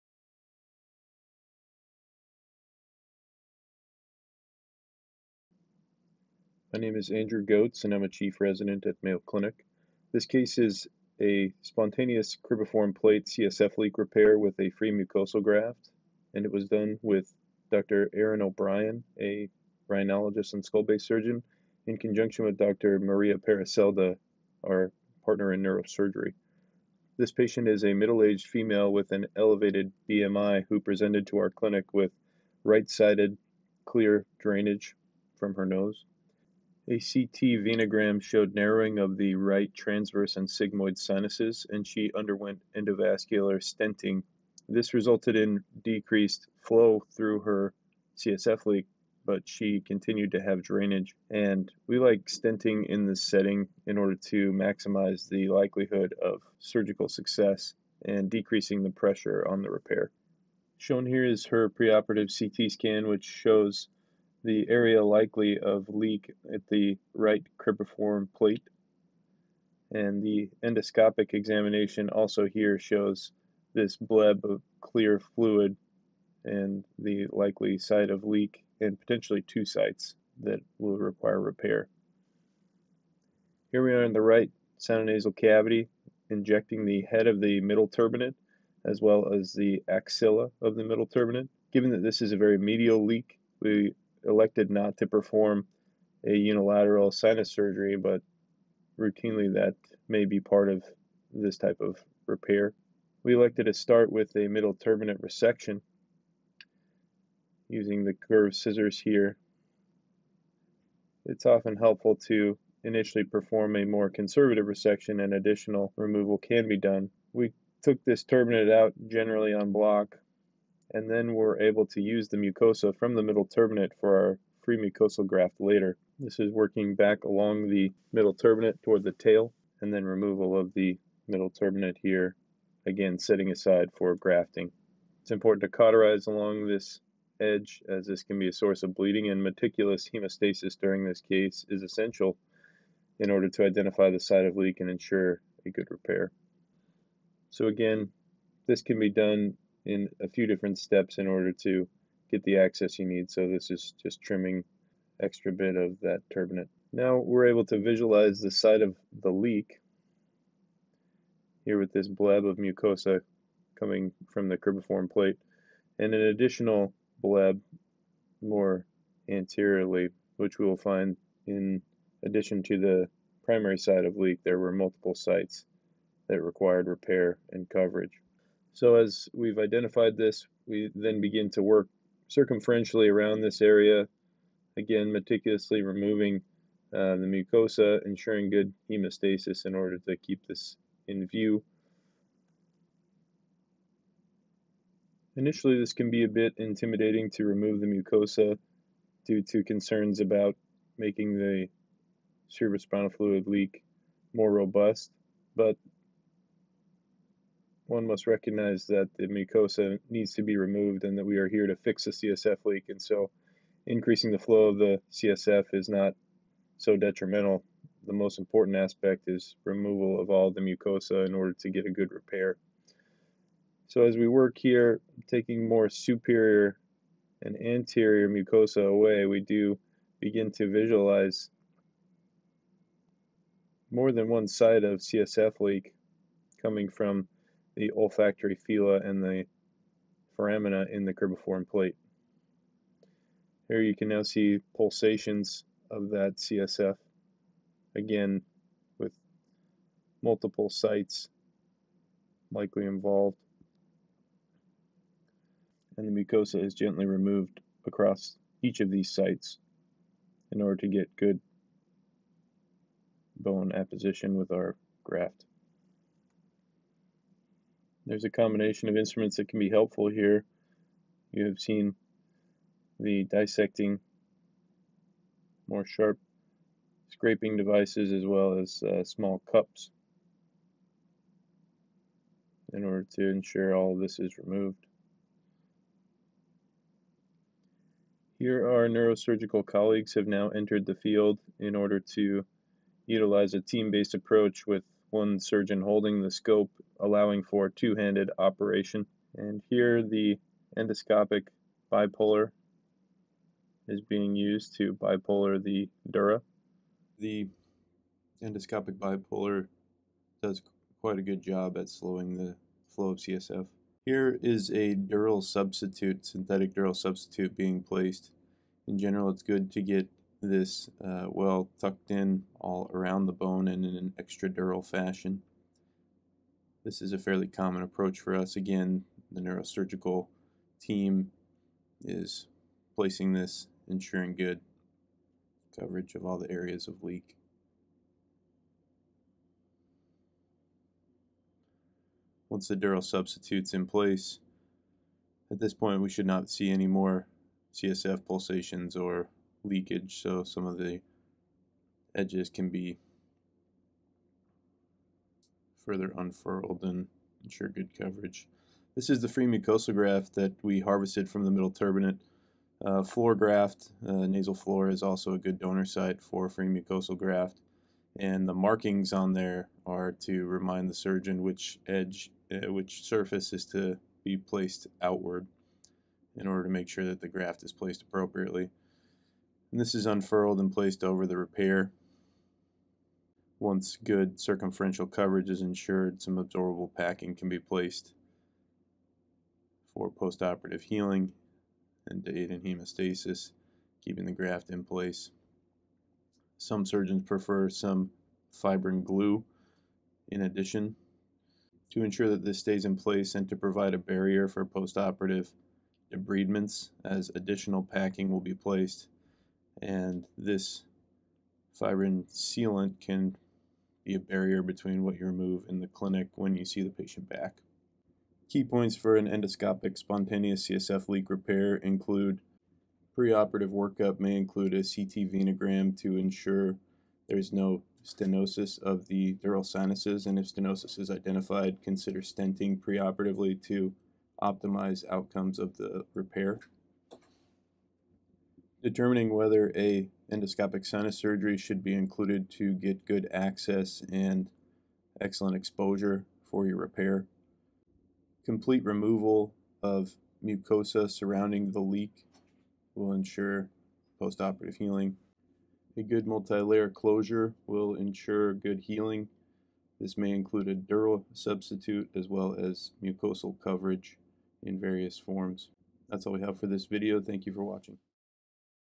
Spontaneous Cribiform Plate CSF Leak Repair with Free Mucosal Graft
Please be advised that this video contains graphic footage of surgery.

This video demonstrates a free mucosal graft repair of a spontaneous cerebrospinal fluid (CSF) leak through the cribiform plate performed by Andrew J. Goates, M.D., Erin K. O'Brien, M.D., and Maria Peris-Celda, M.D. and narrated by Andrew J. Goates, M.D..

Series Editors: Andrew J. Goates, M.D.;  Nicholas R. Randall, M.D.; Matthew L. Carlson, M.D.